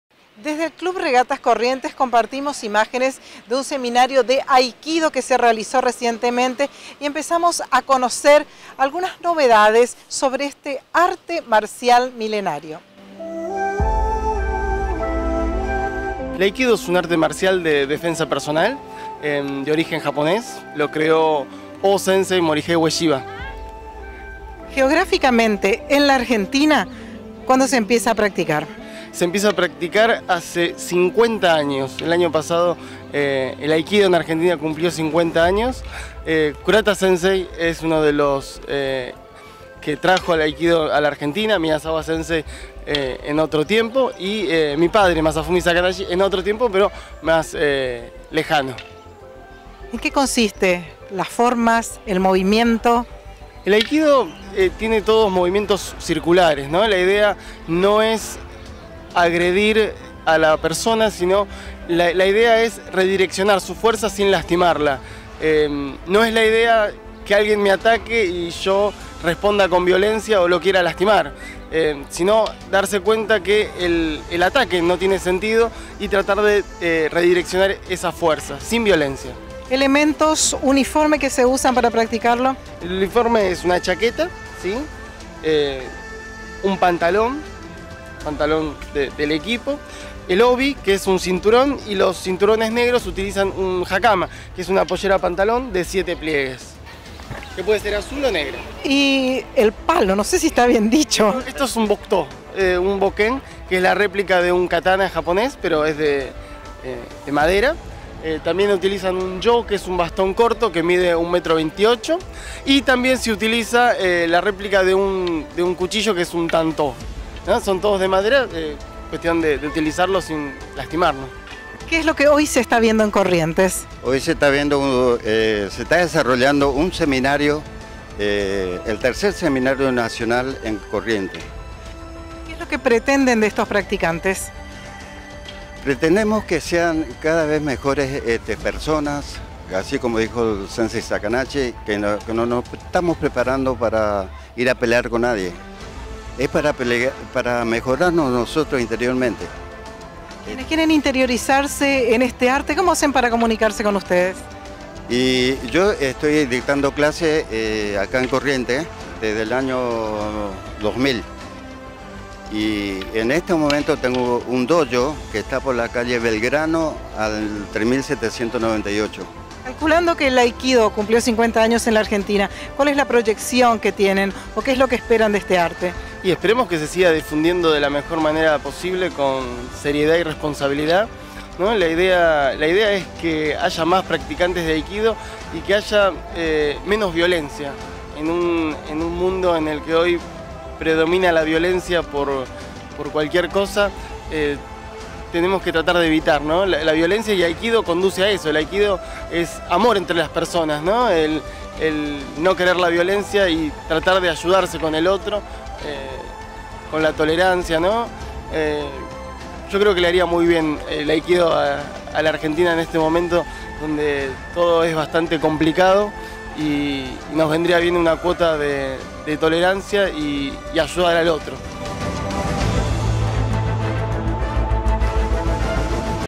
3er Seminario de AIKIDO en Corrientes
Imágenes realizadas en la Playa del Club de Regatas Corrientes . . .